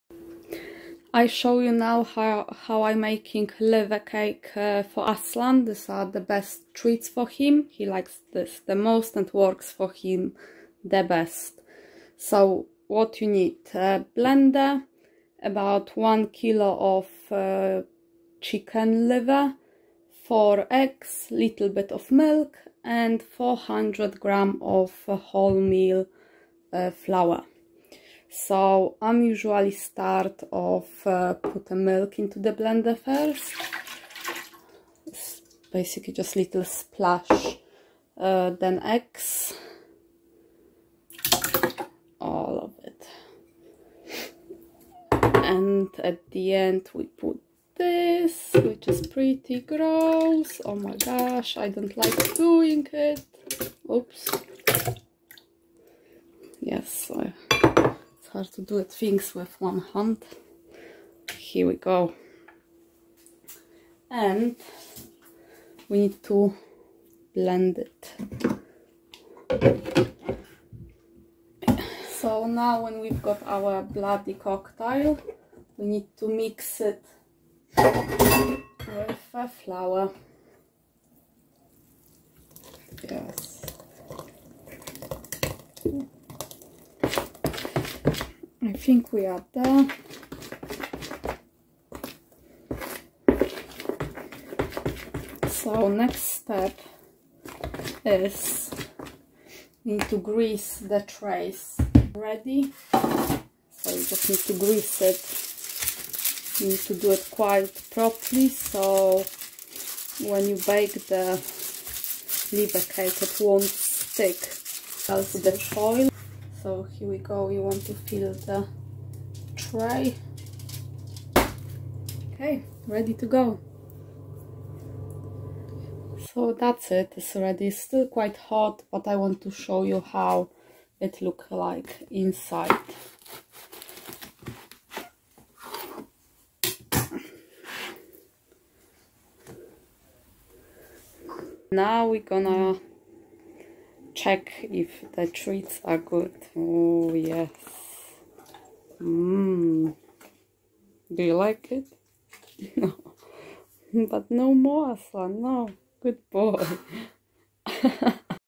How To Make A Liver Cake For Dogs
I make treats for Aslan myself since he is very fussy. This liver cake is a simple and quick treat to make. It is a good treat to use for training and enrichment purposes.

Here are the steps I took and what you need:

A blender, a bowl, and baking trays.

The ingredients are:

1kg of chicken liver
4 eggs
400g wholemeal flour
A half-glass of milk.

Blend the milk, eggs, and liver until smooth.
Once the mixture is mixed with the flour, it is ready to bake.
Pour the mixture into baking trays and bake them for an hour at 200°C in a preheated oven.